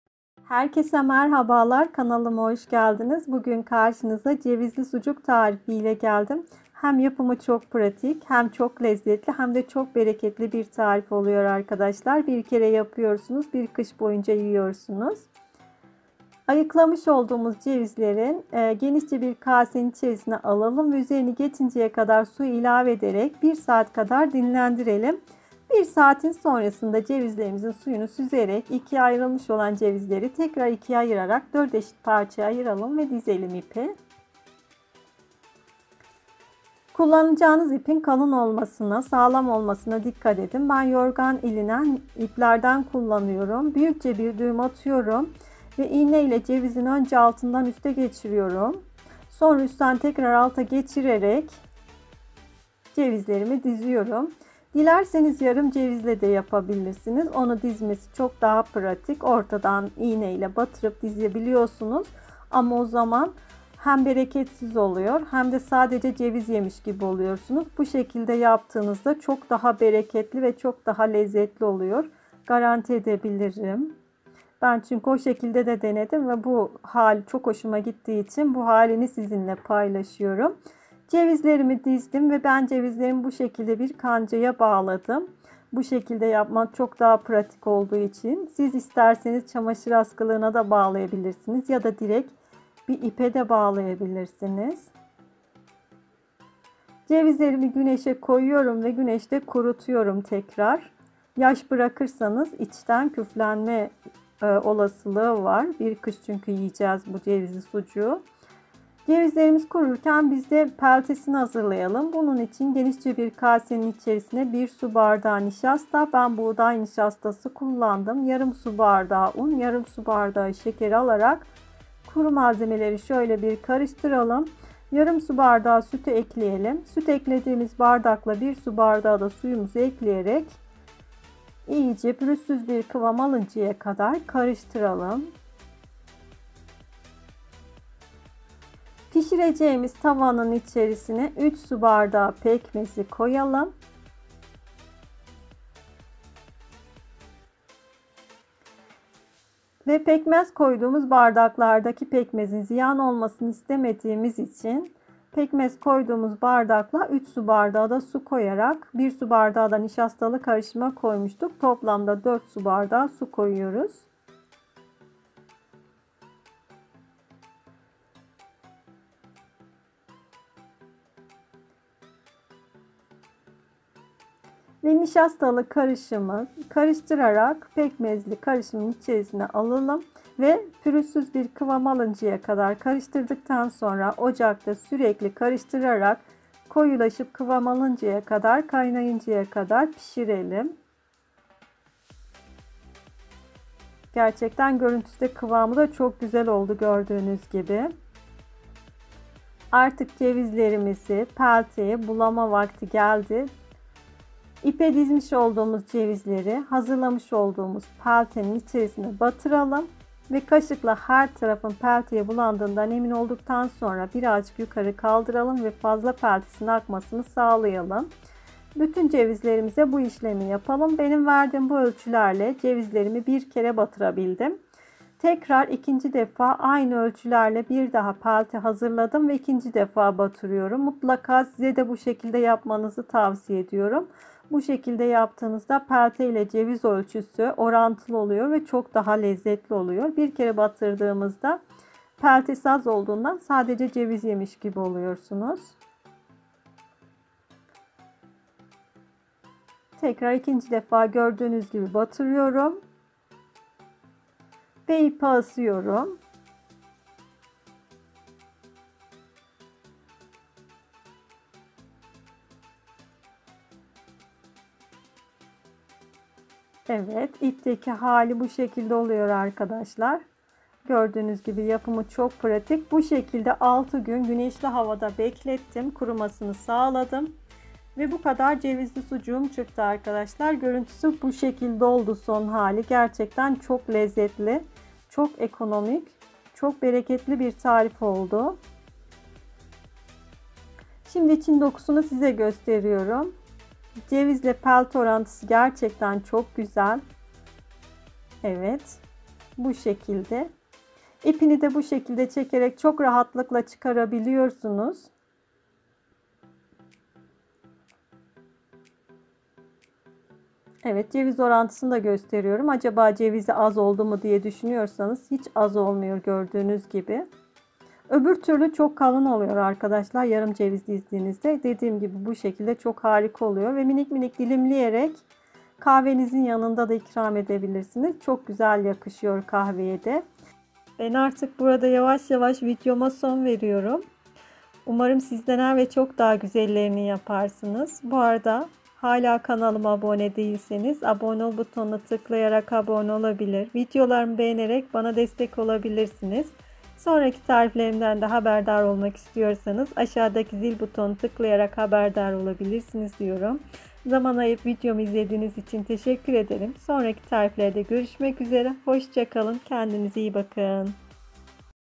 ‼️KAHRAMANMARAŞ IN CEVİZLİ SUCUĞUNU EVDE YAPTIK👌sucuk
CEVİZLİ SUCUK
Malzemeler:
📌Ceviz( 130 adet ceviz kullandım)
📌3 su bardağı pekmez
NOT: Aynı ölçülerle iki defa pişiriyoruz.Benim 30 dal sucuğum çıktı.Bu miktar sizin cevizi dizeceğiniz uzunluğa göre değişir.